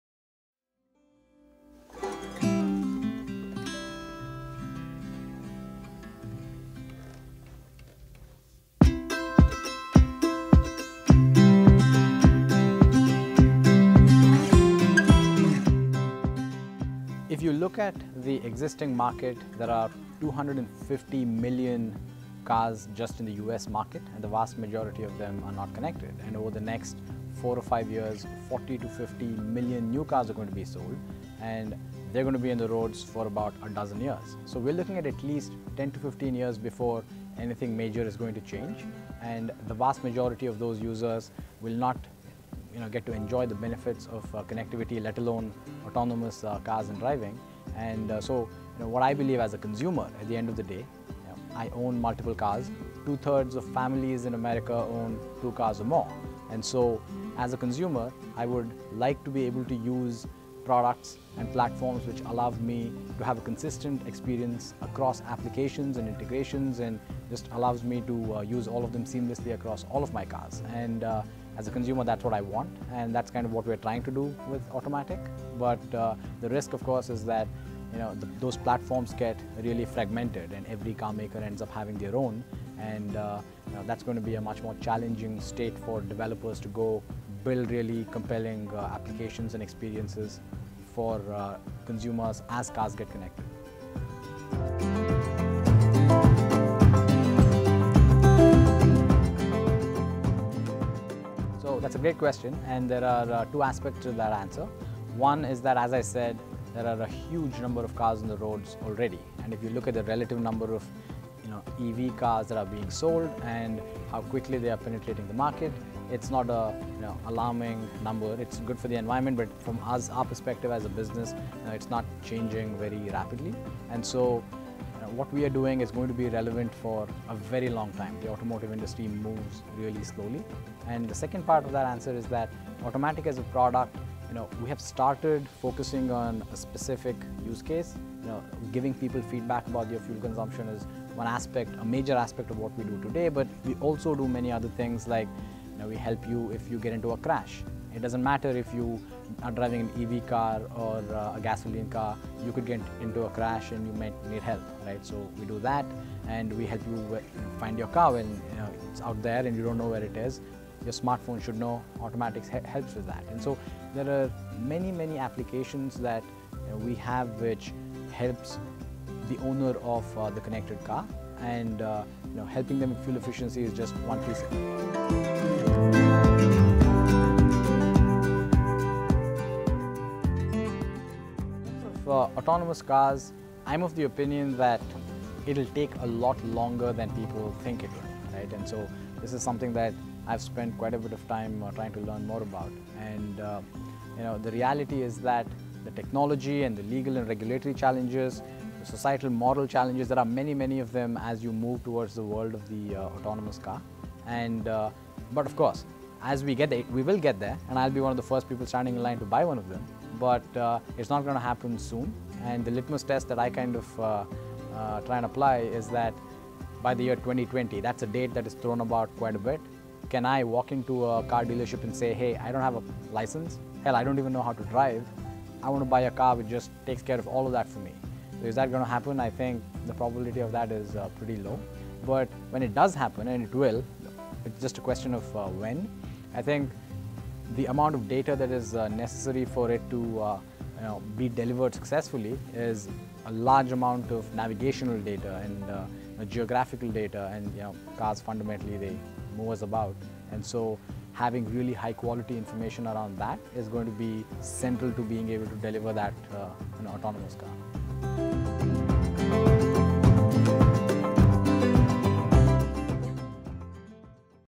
Thejo Kote, Co-founder and CEO at Automatic discusses autonomous and data
Thejo Kote attends as a panelist at @SVForum's event "the Future of Personalized Vehicles" held in collaboration with Deloitte in Palo Alto